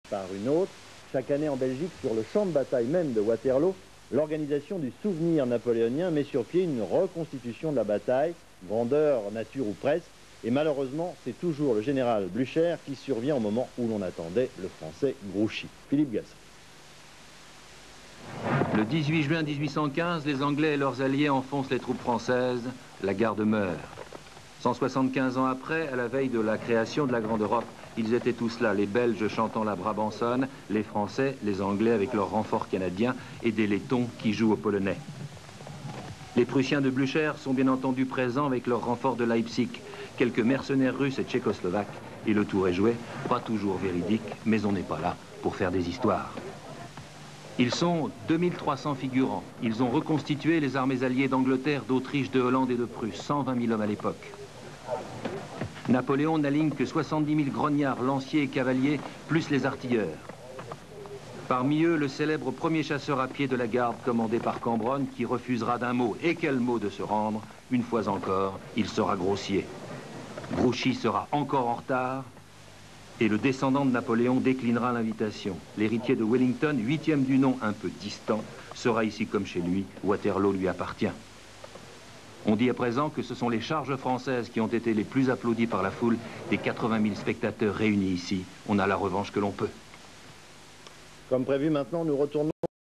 175e anniversaire de Waterloo par la 2 1990
le 175e anniversaire de la bataille de Waterloo en 1815 vu par A2 en 1990

french TV on 175th anniversary of Waterloo battle in 1990